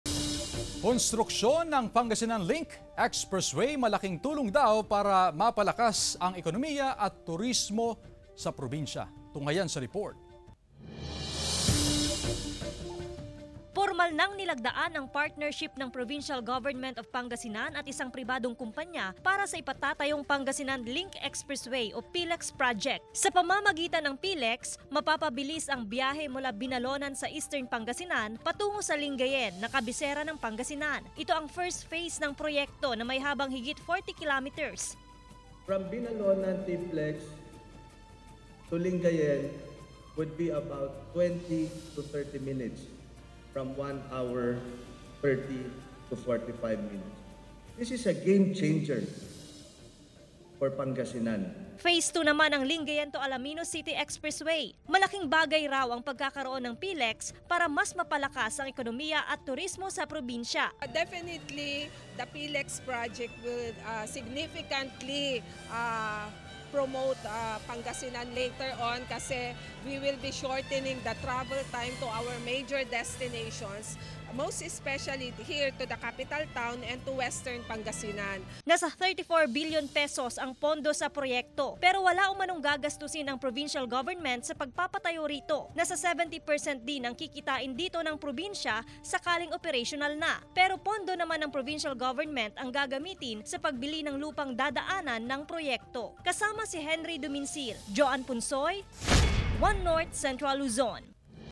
One North Central Luzon: Pagpapatayo ng Pangasinan Link Expressway
Pagpapatayo ng Pangasinan Link Expressway o PLEX, malaking bagay para mapalakas ang ekonomiya at turismo sa probinsya.